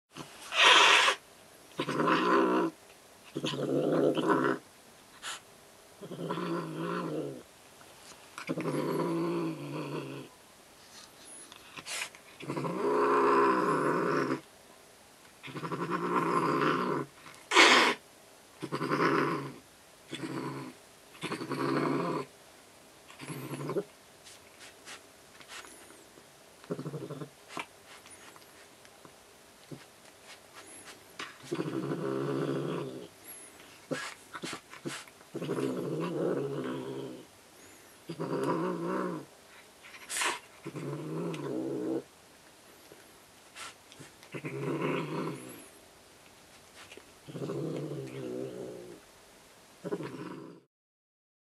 シャーする猫
おもちゃに興奮して怒る猫（おはぎ）です。